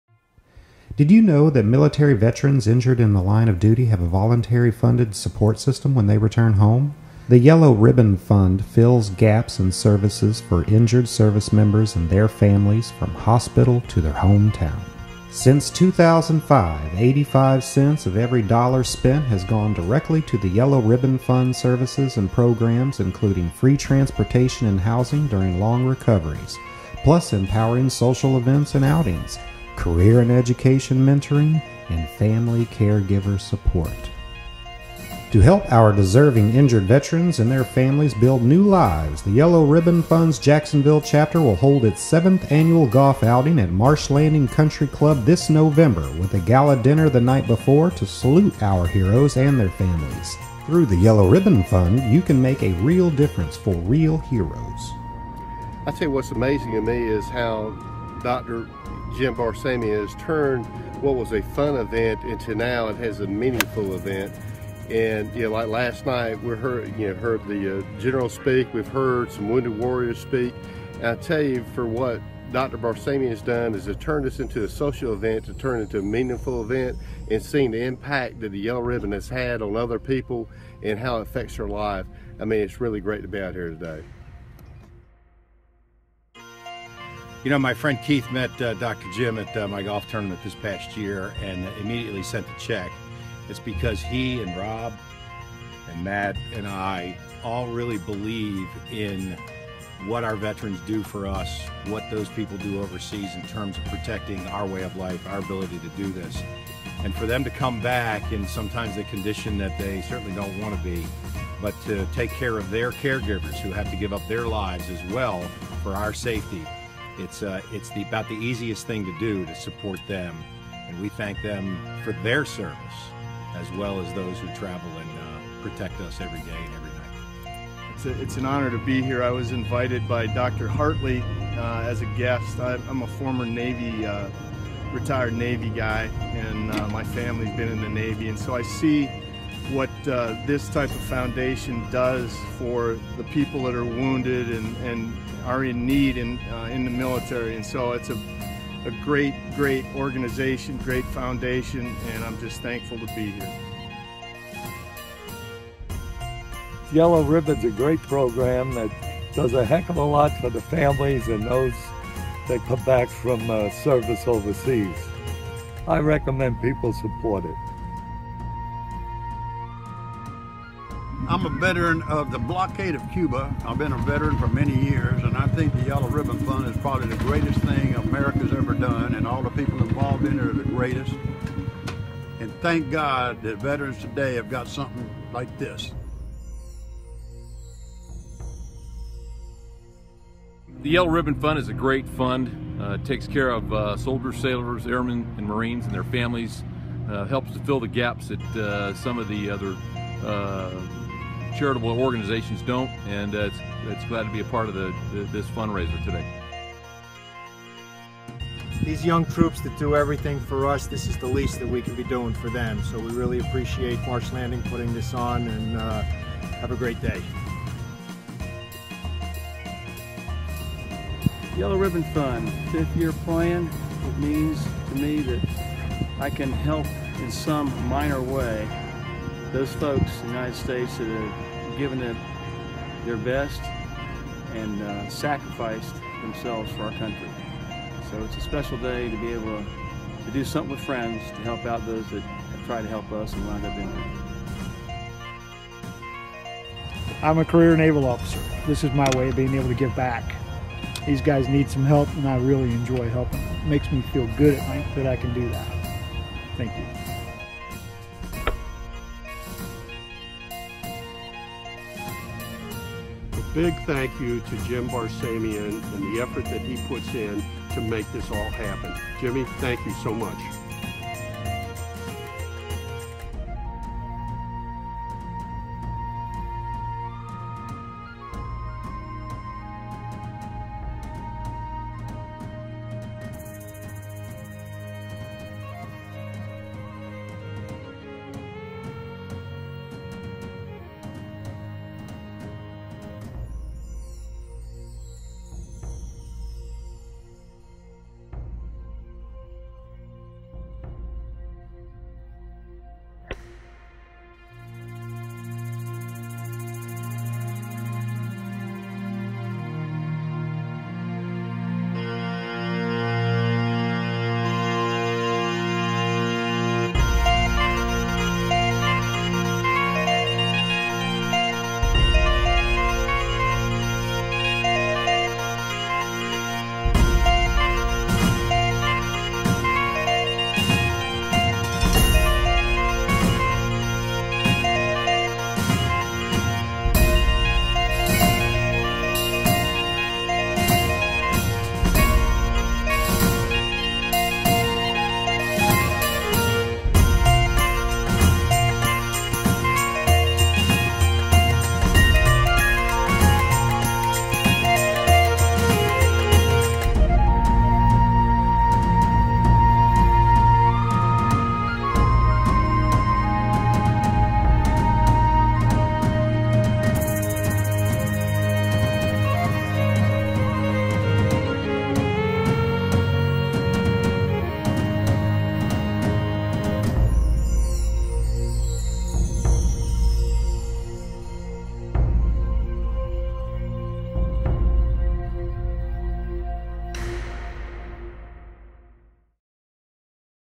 Jacksonville Yellow Ribbon Fund
The Yellow Ribbon Fund listens to what injured service members and their families say they need, and fills those gaps in services, from hospital to hometown. Last year, 85 cents of every dollar spent went to services and programs. At Walter Reed National Military Medical Center in Bethesda, Md., and Ft. Belvoir Community Hospital in northern Virginia, YRF offers free rental cars, taxi rides, hotel stays, and apartments for family members visiting hospitalized loved ones; family-oriented activities, stress-relieving massages, and retreats for family caregivers; plus more than 100 social events and outings yearly, free tickets to sporting and cultural events, and career and education mentoring to help the injured and their families build new lives. Back home, YRF’s Wellness Program and nationwide network of volunteers provide practical support, from job hunts to legal services to accessing benefits. The YRF TUF Team (Taking Up Fitness) of veteran athletes help their fellow veterans find healing through community-based fitness.